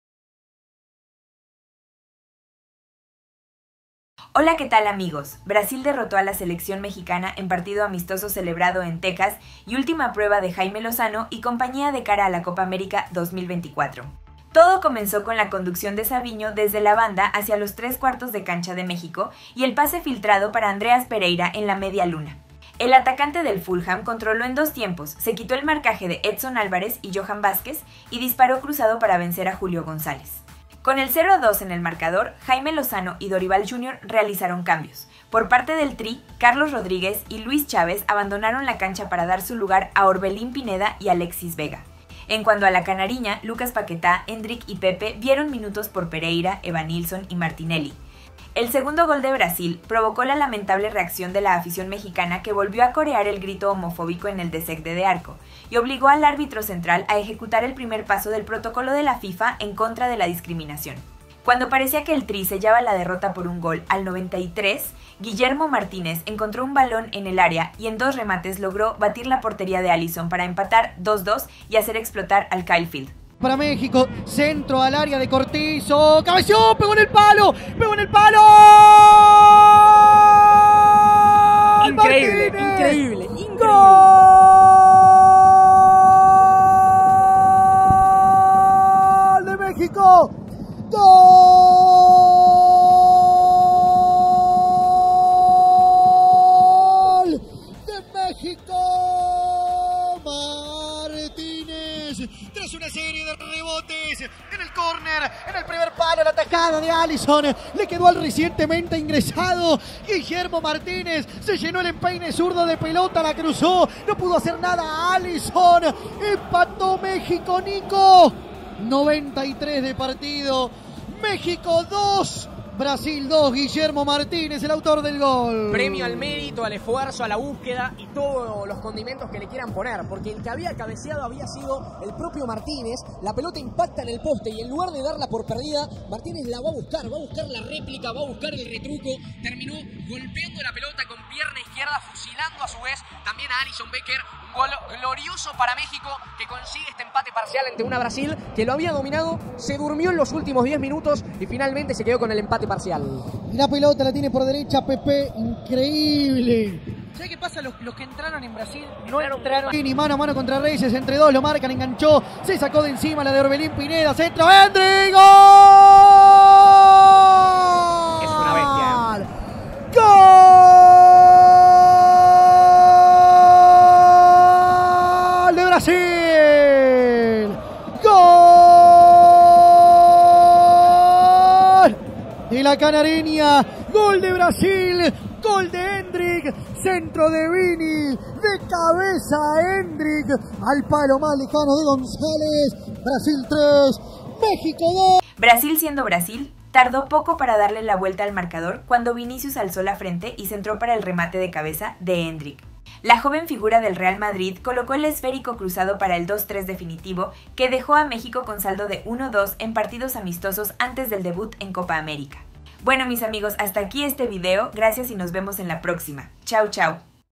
PRENSA ARGENTINA ASOMBRADA CON MEXICO !! MEXICO ES DIGNO !! HOY DEMOSTRÓ QUE PUEDEN CON POTENCIAS
Brasil venció a México 3-2 por amistoso previo a Copa América 2024: formaciones y resultados
Brasil y México se enfrentaron el sábado 8 de junio, en un partido amistoso desde el estadio Kyle Field en Texas a las 18:30 del centro de...
Apareció la “joya”: Endrick marcó el 3-2 final de Brasil vs. México en el último minuto | VIDEO
Endrick le dio la victoria a Brasil contra México en el partido amistoso FIFA antes de la Copa América.

Dónde ver Brasil vs México en Perú: Tv y cómo seguir choque por la fecha FIFA 2024

Las selecciones de Brasil y México se medirán, hoy, sábado 8 de junio en un amistoso a disputarse en el estadio Kyle Field de Texas,...

México vs Brasil hoy: A qué hora juegan, dónde ver, historial y pronóstico partido amistoso hoy 2024